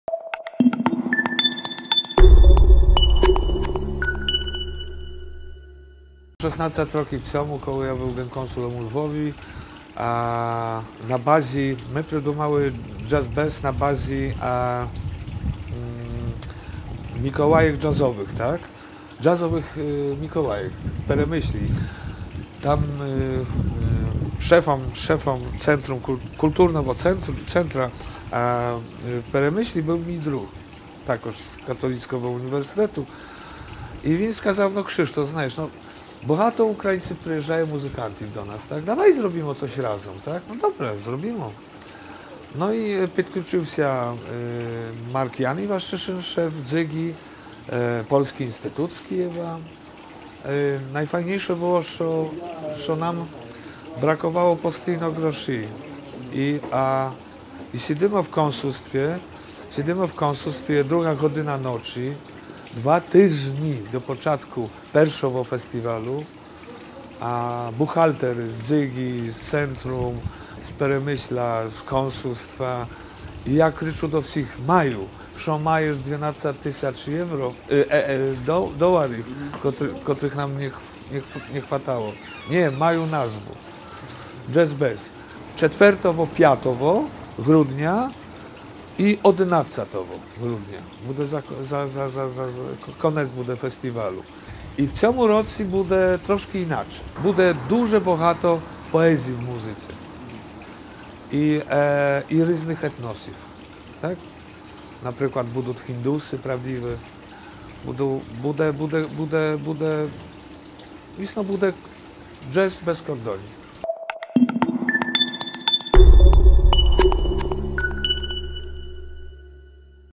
У Луцьку джазмени руйнуватимуть кордони
Уже восьмий рік поспіль Луцьк прийматиме джазовий фестиваль «Jazz Bezz». Імпреза відбудеться 3 – 4 та 11 грудня в луцькому Театрі ляльок.
«В цьому році буде трохи інакше. Буде багато поезії в музиці. Також на фестивалі виступатимуть представники різних етносів. Будуть справжні індуси, буде.. буде … буде джаз без кордонів», - зазначив співзасновник та співорганізатор фестивалю Кшиштоф Савіцкі.